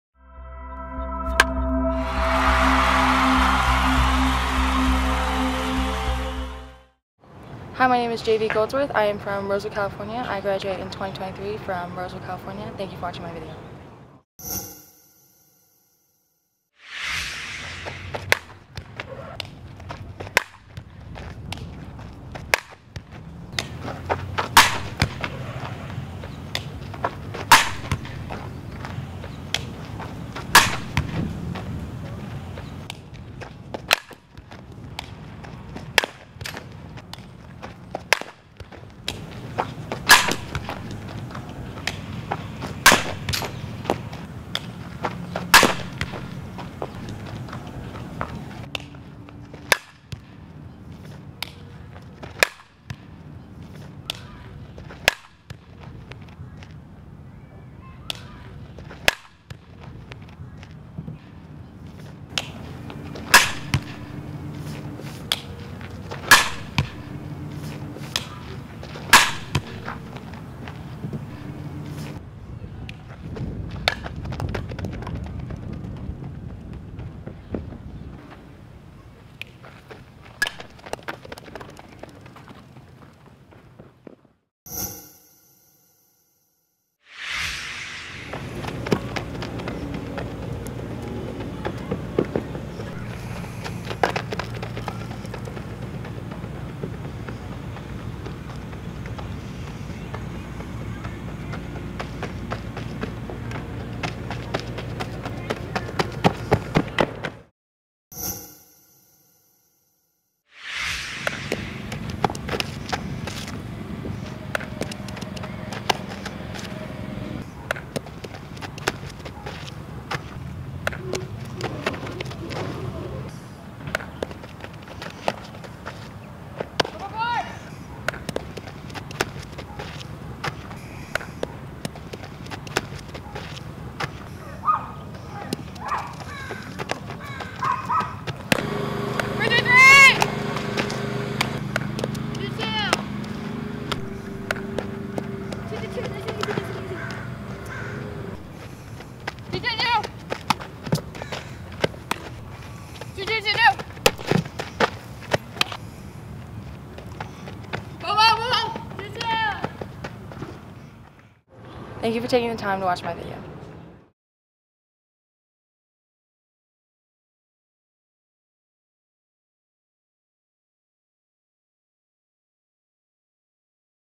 2023 Jennavieve Goldsworth Committed Fort Lewis Athletic Lefty Slapper & SS Softball Skills Video
2023 Jennavieve Goldsworth 3.9 GPA - Athletic Lefty Slapper Shortstop Softball Skills Video - Ca Grapettes Hollingsworth